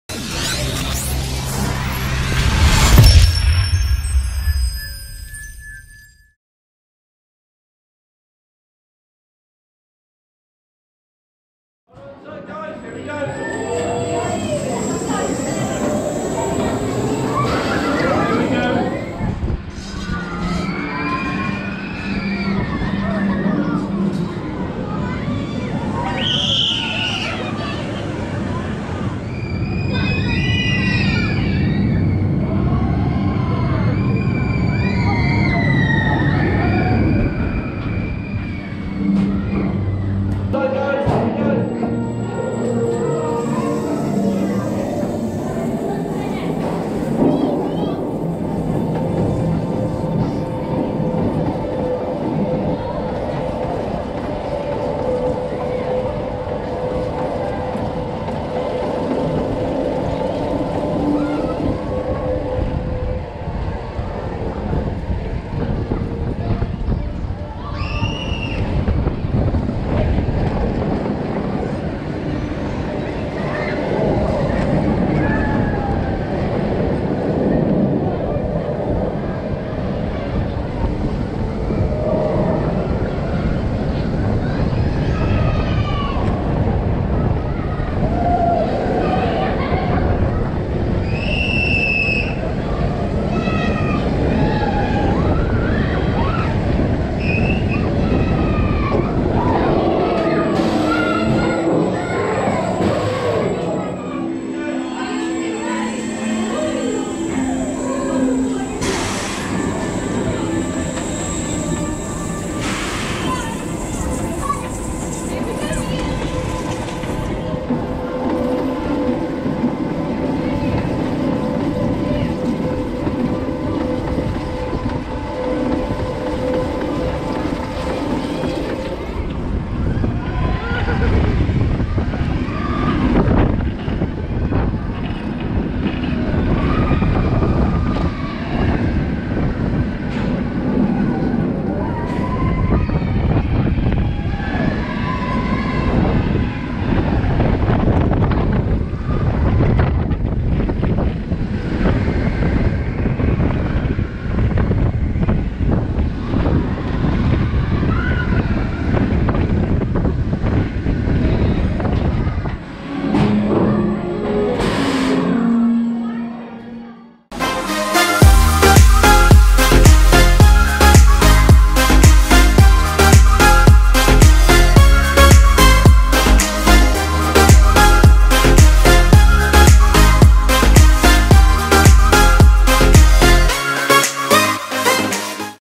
Flight of the Pterosaur 4K Front & Back Seat POV - Paultons Park
Take a ride on Flight of the Pterosaur which is located at Paultons Park, UK.

- XtremeCoasters Network -